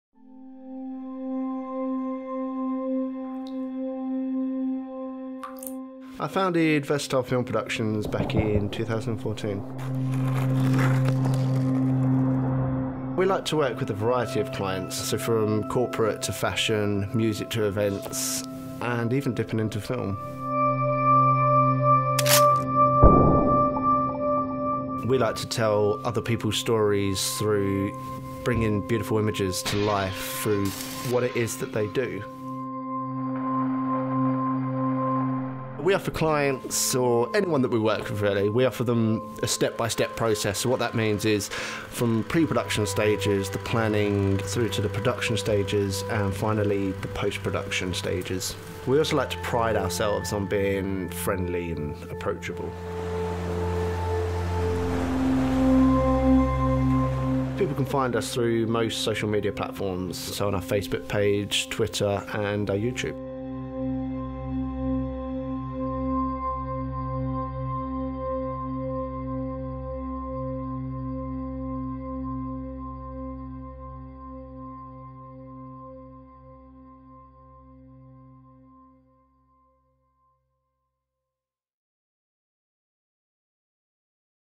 Versatile Film Productions - About Us (Short Ad)
Versatile Film Productions (VFP) was founded in 2014, with much creativity, VFP creates a variety of films from Corporate to Fashion, from Music to Events, we even work in short & feature films.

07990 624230

Film by Lee Bethell
Production: Versatile Film Productions

Other Credits:
Mitch Martinez - Videographer
Videezy.com
Vimeo